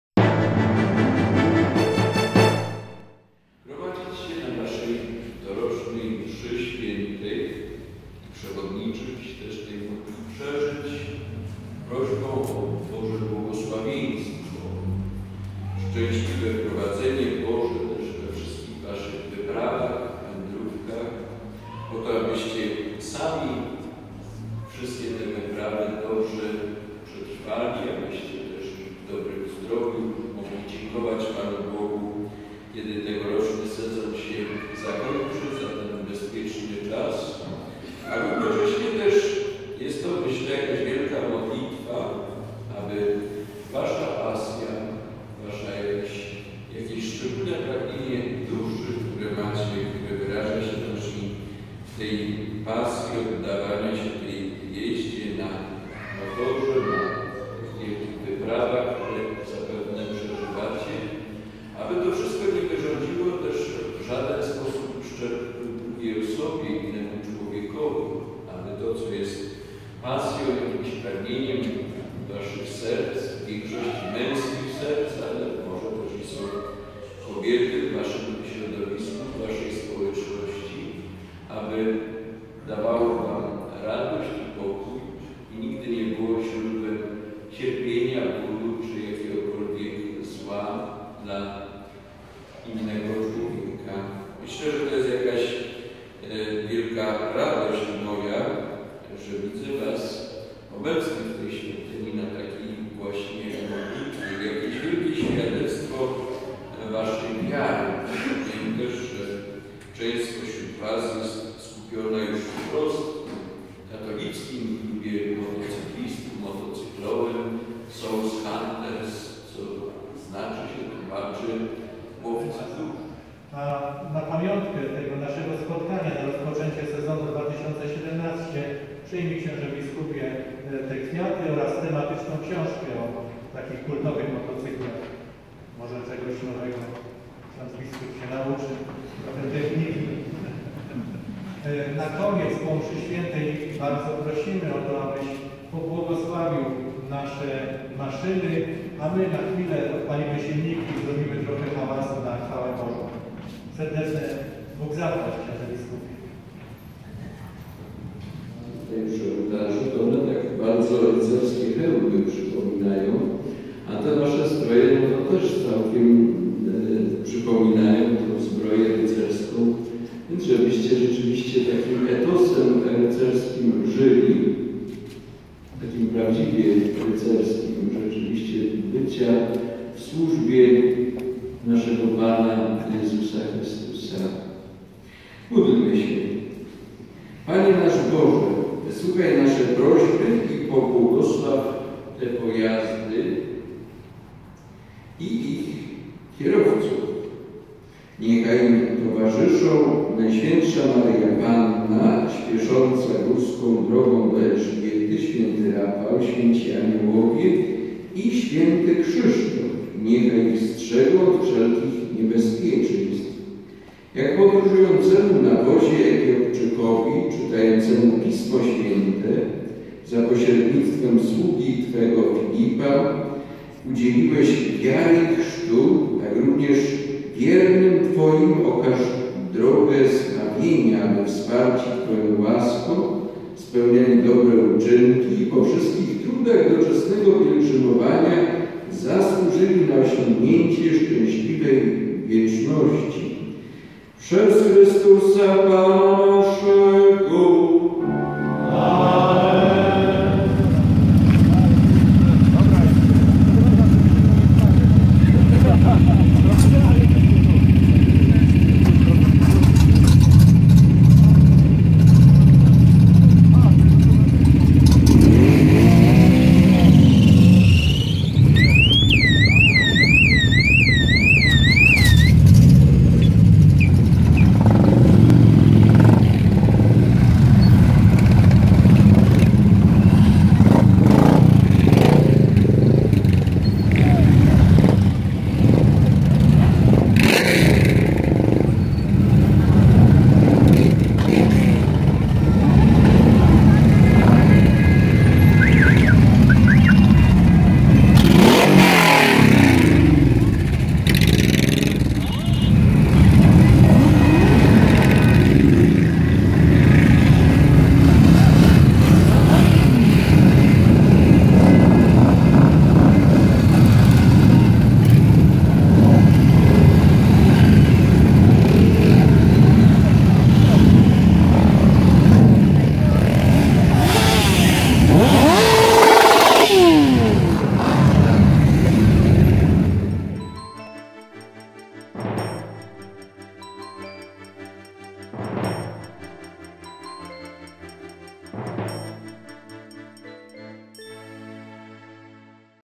Żory: msza dla motocyklistów na rozpoczęcie sezonu
Od wspólnej modlitwy podczas Mszy świętej w kościele pw. Miłosierdzia Bożego rozpoczęli (22.04) nowy sezon motocykliści z całego Śląska. Eucharystii przewodniczył biskup Adam Wodarczyk, który również pobłogosławił kierowców i ich jednoślady. Po mszy motocykliści przejechali ulicami Żor i swoje spotkanie zakończyli na placu przy Komendzie Miejskiej PSP. FOTO